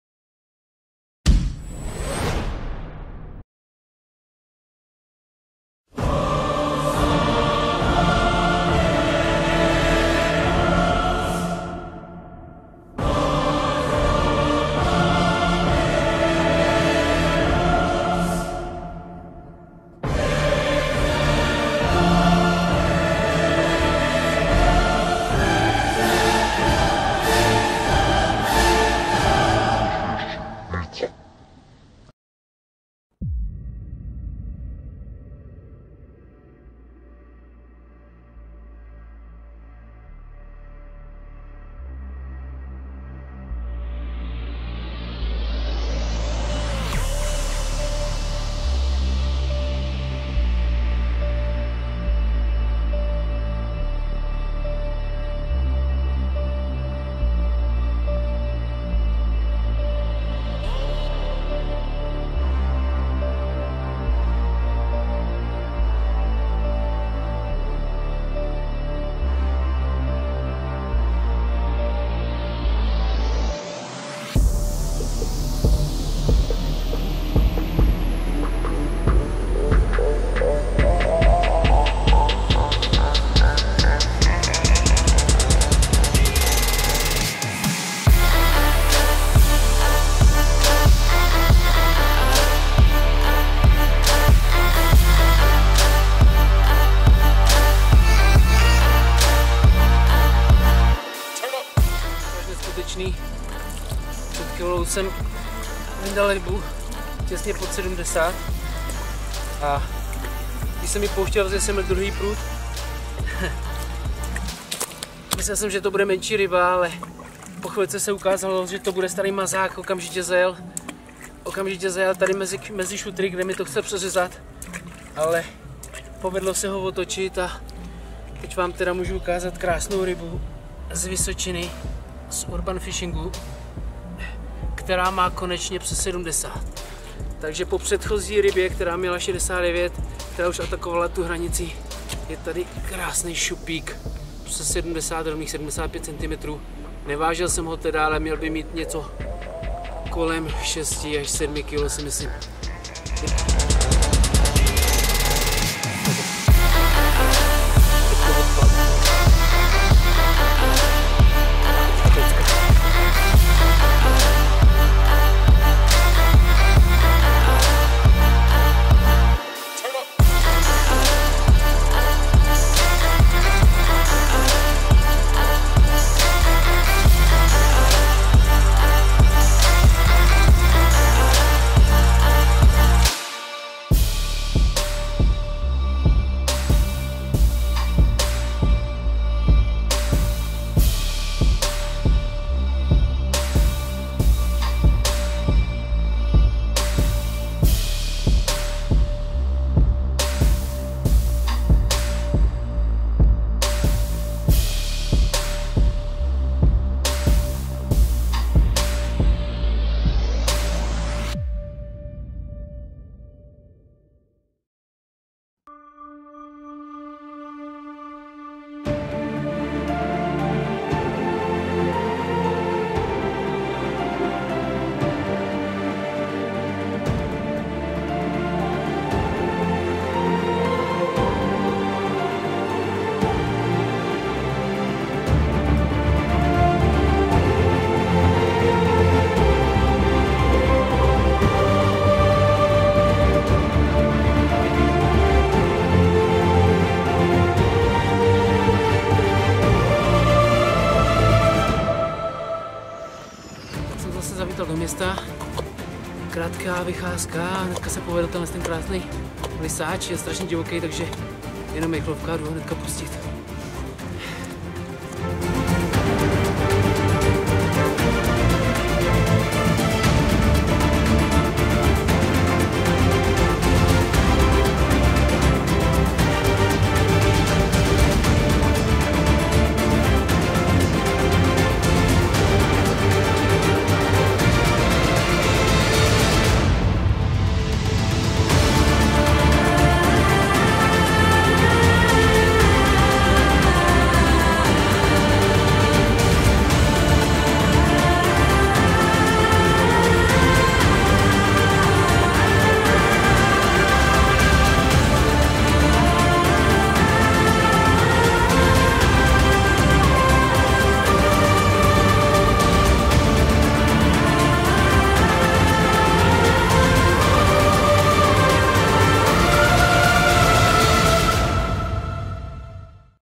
Urban fishing na Vysočině_ Summer 2020 - VOL. 2
Hlavním cílem letošní rybářské sezóny byl takzvaný URBAN FISHING (lov ryb v městském prostředí), který jsem se rozhodl zdokumentovat v podobě videozáznamu. Vše se odehrálo v průběhu letních měsíců, kdy jsem bohužel zvládl jen 8 kratších vycházek k vodě. Probádal jsem youtube a google zda vůbec někdy někdo podobné video na Vysočině natočil, ale nic jsem nenašel. S potěšením vás tedy zvu k vůbec prvnímu ,,urbanfishingu" z prostředí Vysočiny v podobě videa, které je rozdělené do dvou dílů.